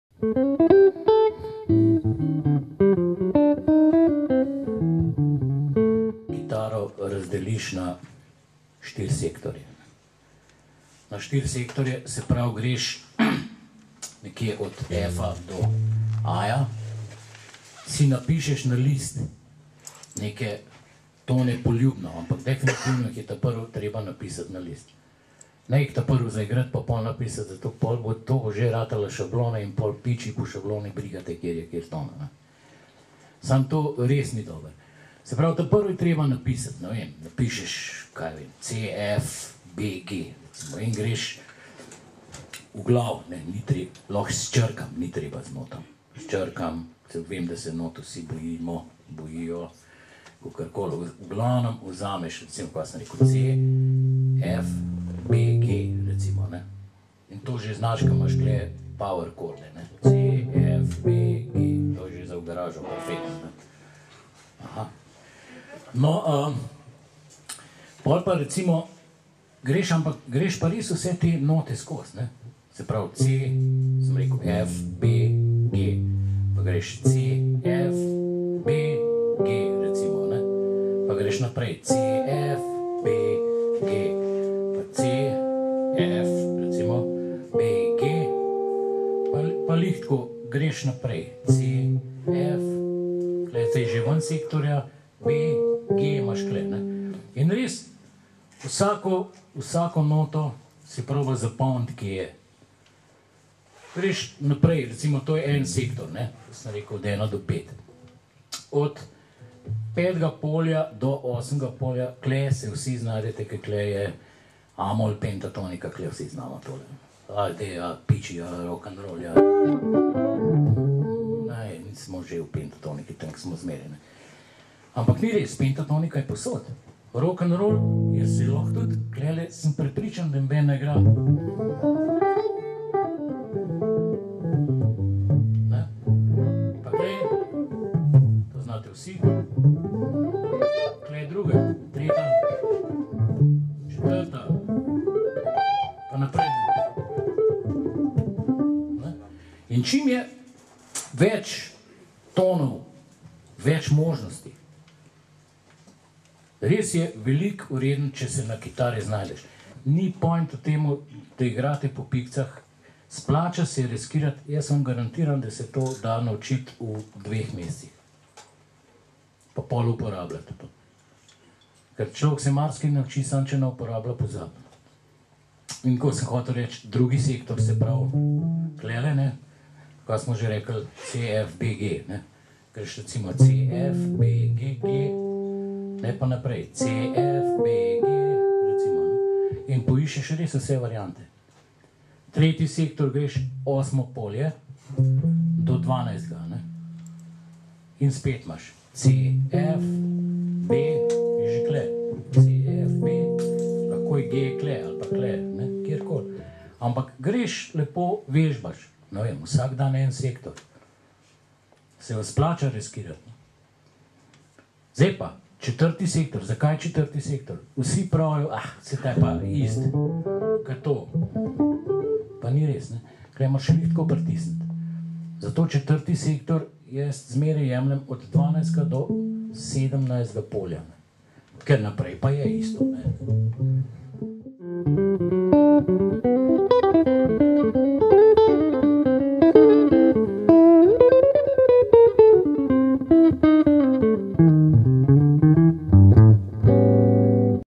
Nasveti za učenje tonov na kitari | Primož Grašič | Zgodba o kitari
Primož Grašič govori o učenju tonov na kitari - Glasbena klinika Zgodba o kitari, 7.5.2013 Trgovina MUSIC MAX v Ljubljani

Prav tako lahko vidite, kako zaigrati pesem. Video vam pomaga pri učenju kitare.